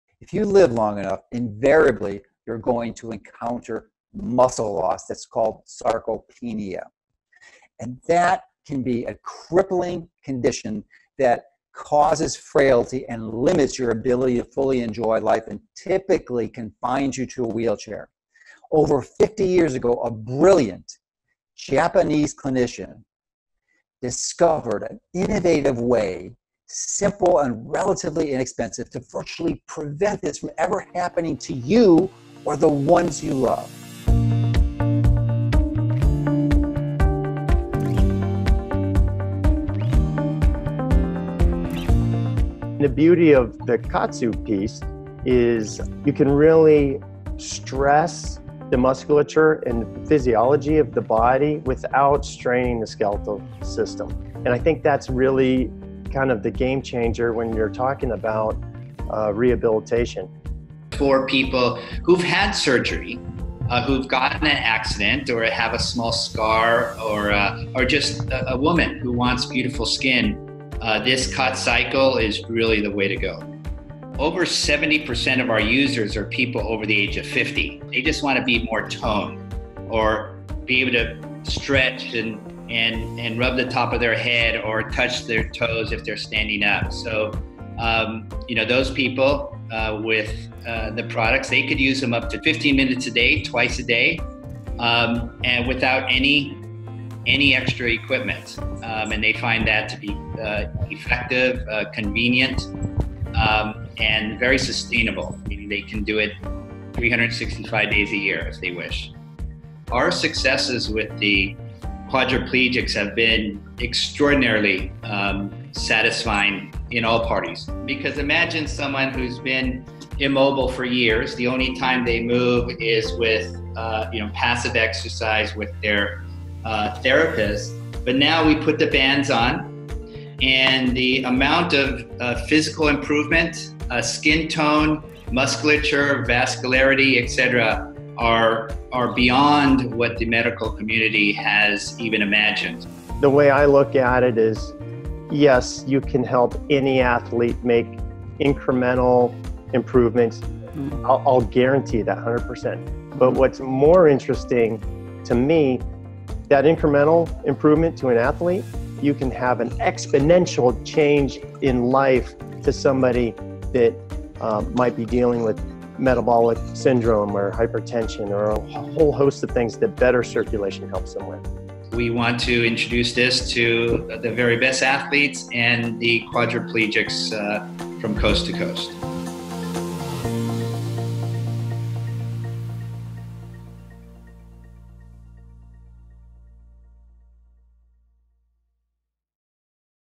Los ejercicios de restricción del flujo sanguíneo para conservar la masa muscular
El entrenamiento de restricción del flujo sanguíneo (BFR) consiste en una restricción parcial de la entrada arterial y restricción total de la salida venosa, mientras ejercita el músculo.